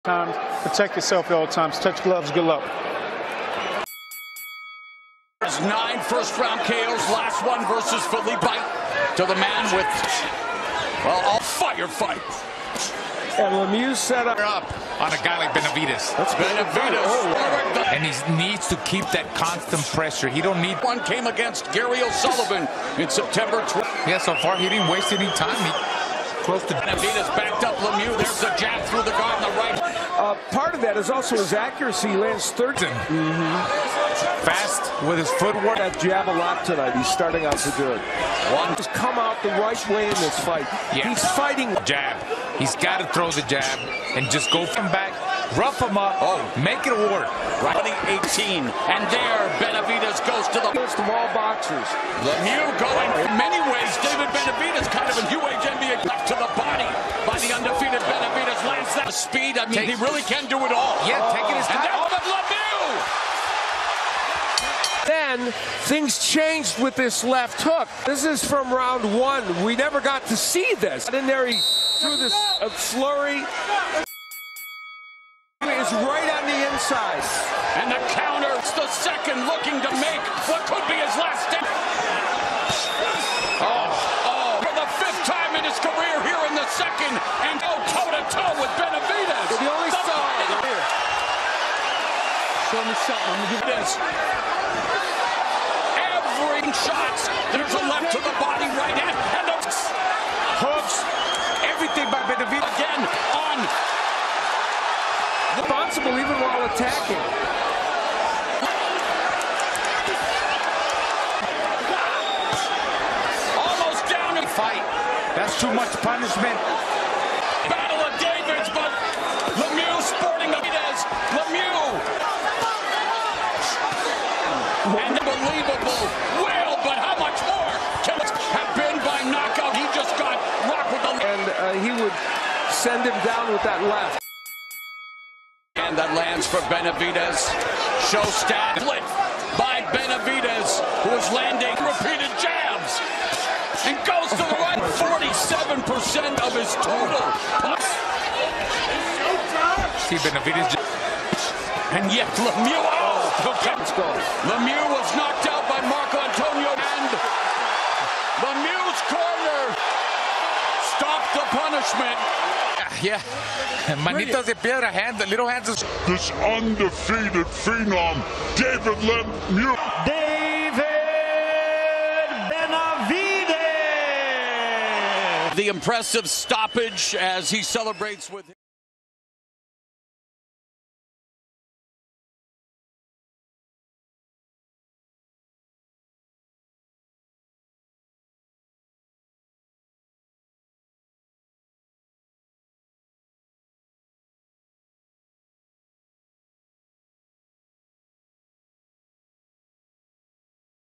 David Benavidez vs David Lemieux | Fight Highlights | BOXING FIGHT | HD
Watch the explosive showdown between David Benavidez and David Lemieux as two power-punchers clash in the ring! Benavidez, known for his relentless pressure and devastating combinations, takes on the hard-hitting Lemieux, who’s eager to prove his place among the elite. This fight is packed with nonstop action, brutal exchanges, and skillful maneuvers, as both warriors refuse to back down. The highlights capture every thrilling moment, from Benavidez's sharp jabs to Lemieux's powerful counters. Who will land the knockout blow? Don’t miss out on this epic battle between two of the toughest fighters in the division!